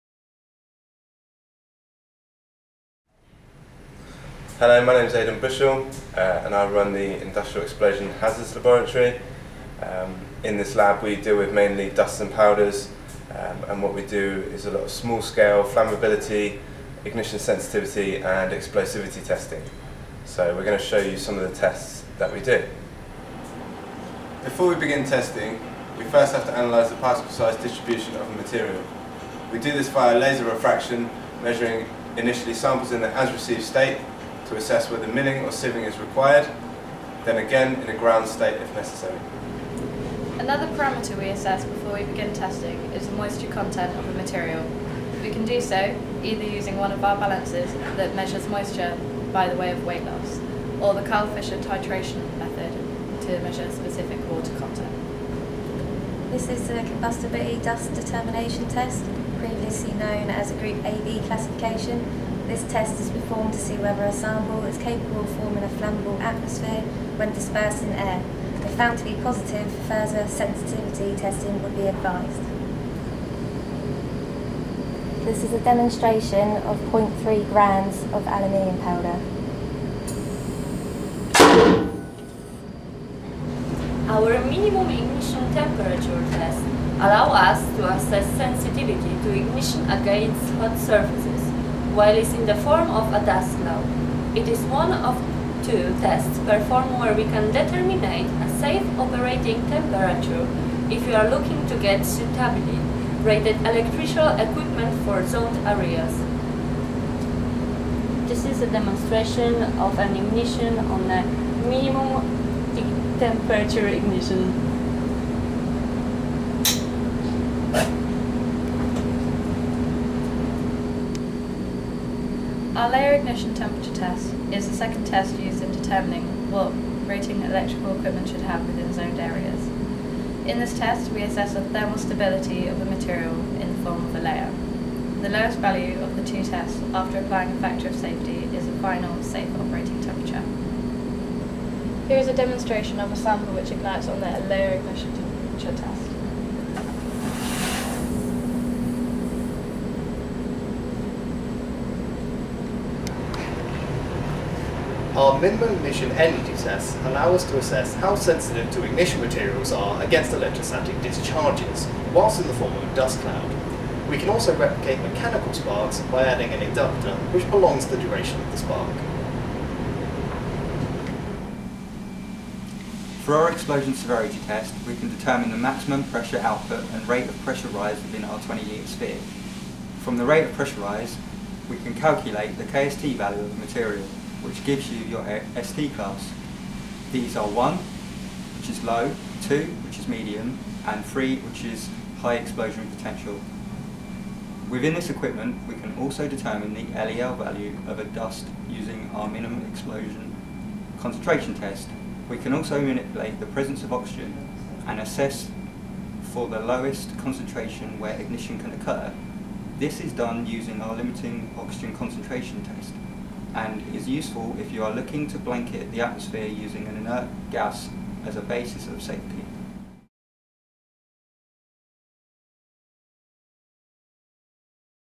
Industrial Explosion Hazards Laboratory – DEKRA Process Safety
Welcome to our DEKRA Industrial Explosion Hazards (IEH) laboratory, where we conduct flammability tests and study the thermo-properties of materials.

In this video tour, we are going to show you some of the tests we perform in our industrial explosion hazards laboratory aimed at assessing the sensitivity of materials to ignition. With these tests we can also determine safe operation temperatures for certain production processes, as well as the requirements for safe equipment for the production areas.

00:24 – Particle size distribution analysis (prior to testing)
00:40 – Analysis of moisture content (prior to testing)
00:57 – Combustibility dust determination test
01:24 – MIT (Minimum Ignition Temperature) test
02:02 – Layer Ignition Temperature Test
02:35 – Minimum Ignition Energy Test
02:54 – Explosion Severity Test

DEKRA Process Safety –Trusted Advisors for Process Safety Excellence